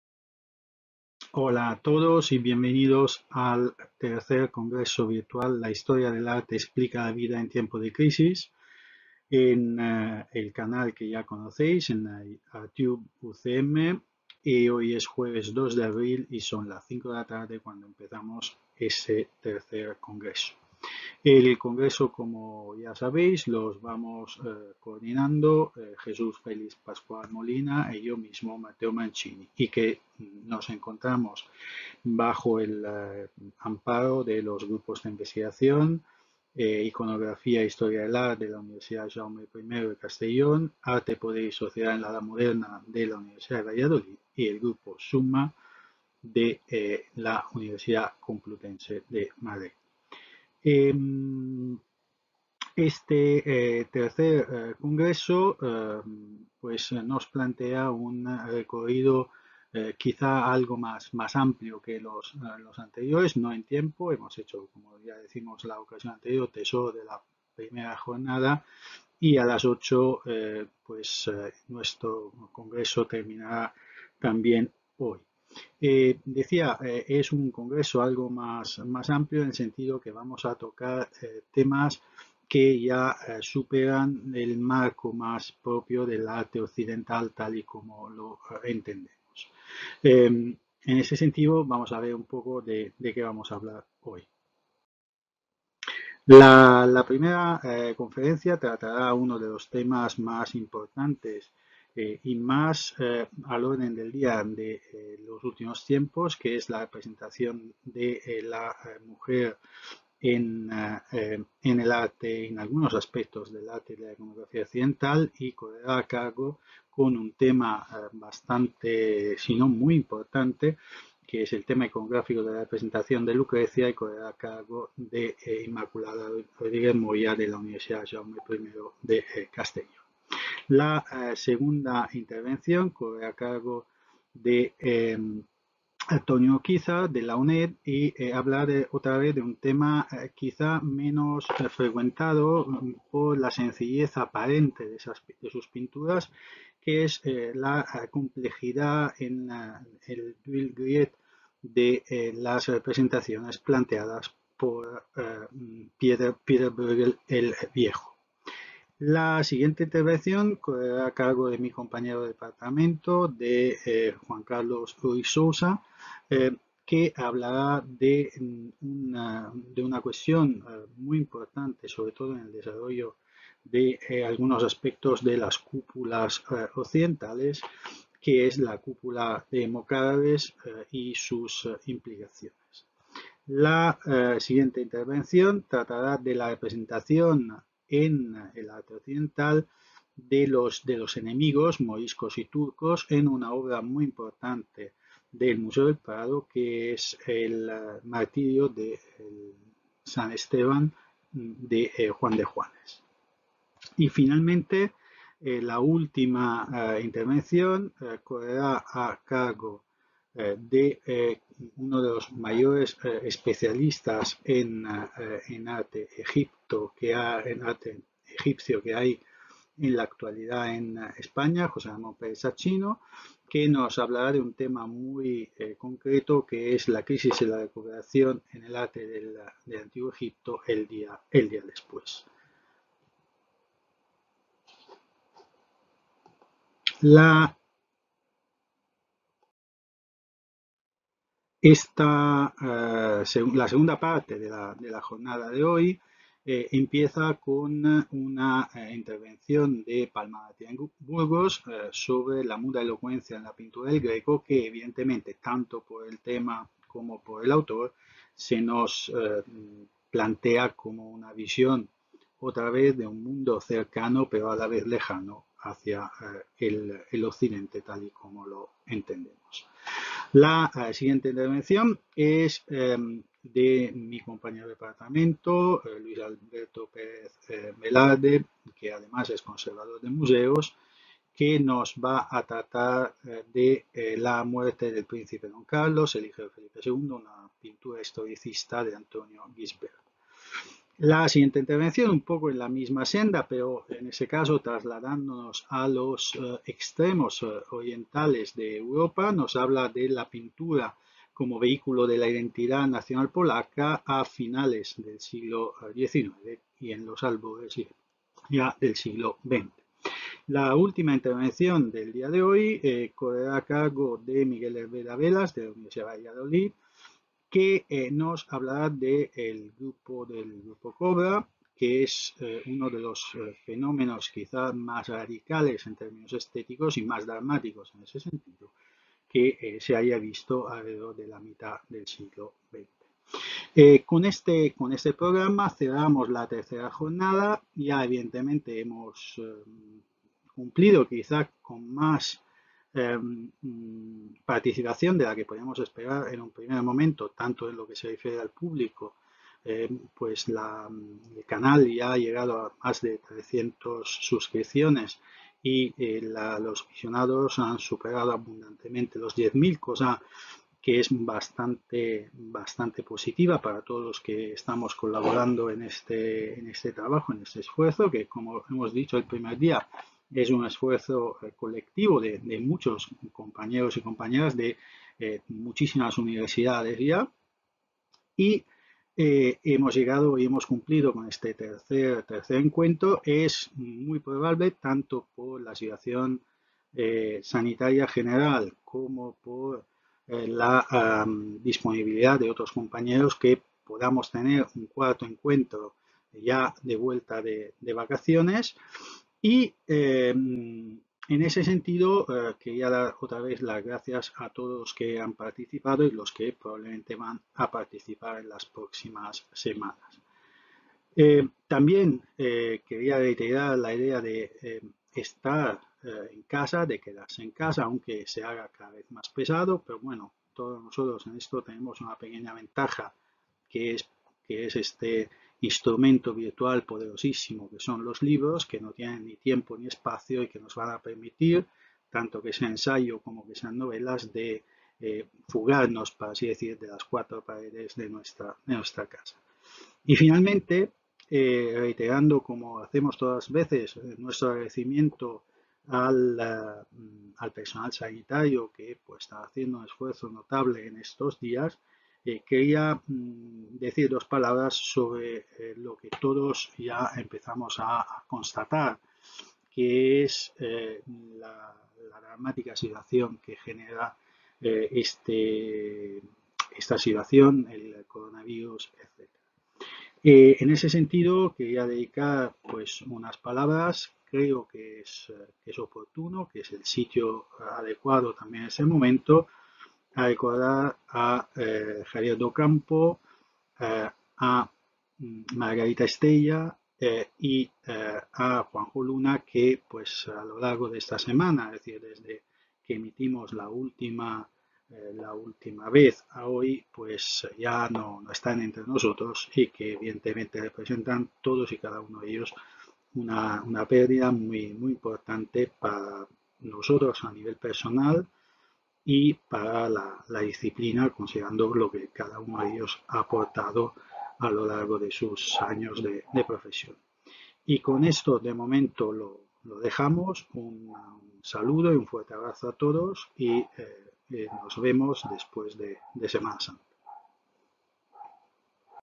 Presentación III congreso La Historia del arte explica la vida en tiempos de Crisis, M.Mancini (UCM)
Presentación III congreso Internacional La Historia del arte explica la vida en tiempos de Crisis, por Matteo Mancini (UCM).